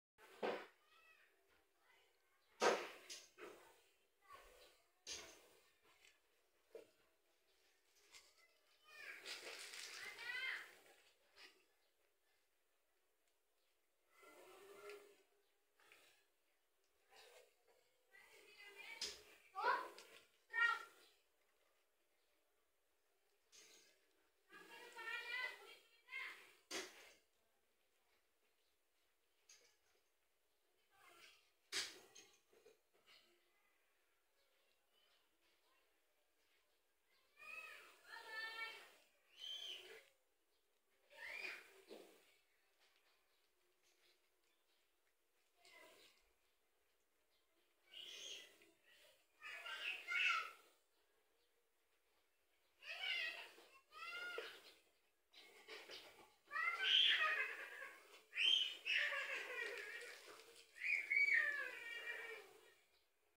HOW MOMMY CAT CARES HER BABY IN DAY 5
BABY CAT IN DAY 5 LITTLE EYE OPEN